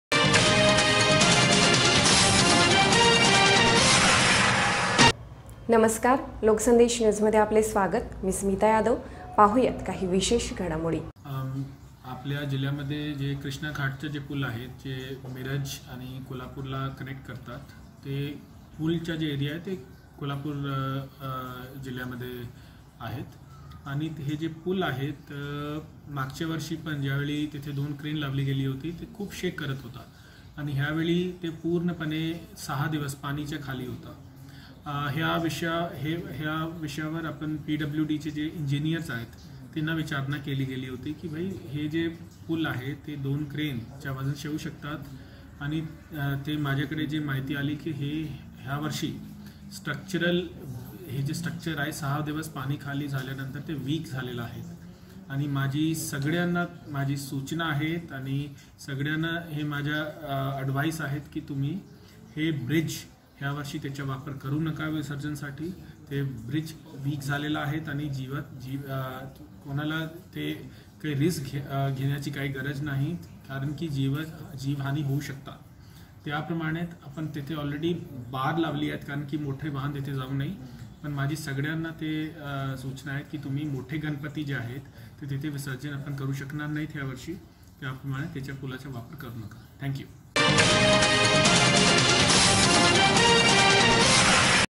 मिरजेतील कृष्णाघाट येथील पूल कमकुवत झाल्याने पुलावरून गणपती विसर्जन करू नका
मिरजेतील कृष्णाघाट येथील पूल कमकुवत झाल्याने पुलावरून गणपती विसर्जन करू नका असे आवाहन सांगली जिल्ह्याचे पोलीस अधीक्षक सोहेल शर्मा यांनी केले.